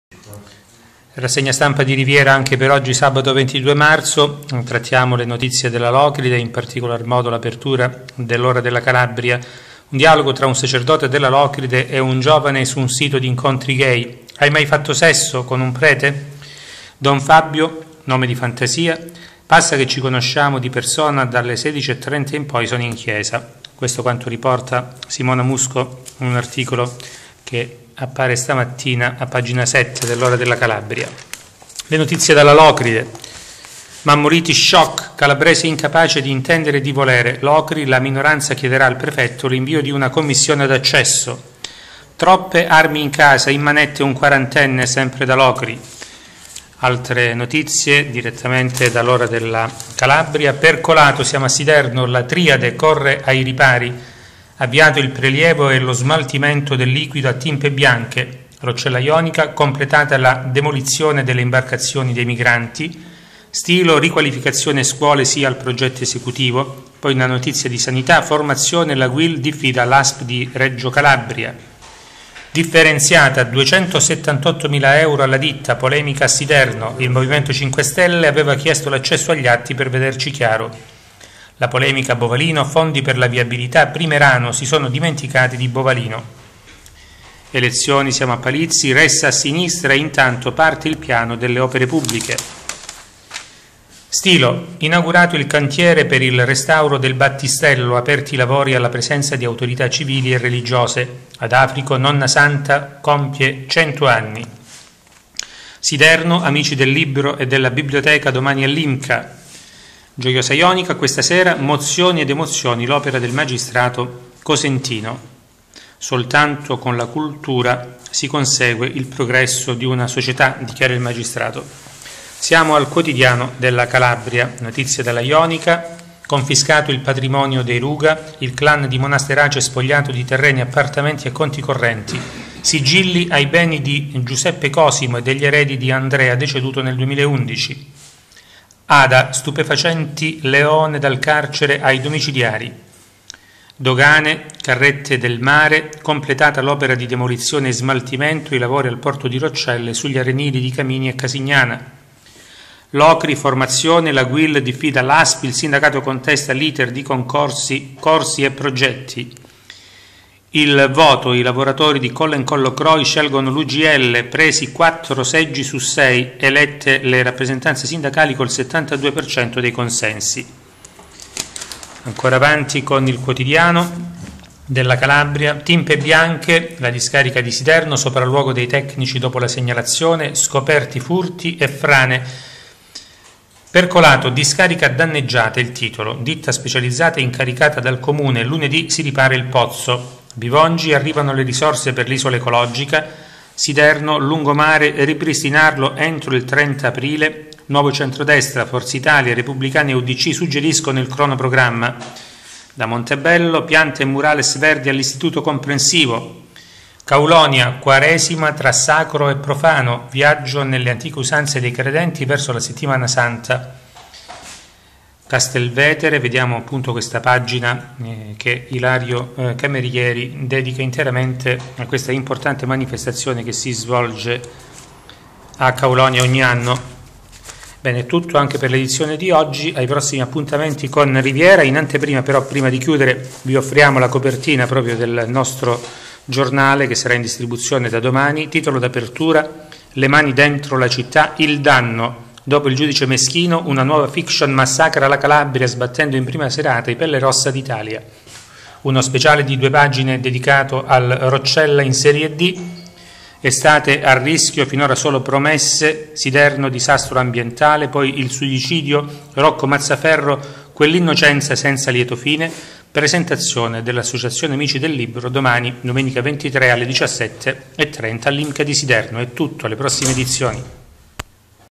Rassegna stampa 22/03/14
Il direttore Antonio Tassone legge i quotidiani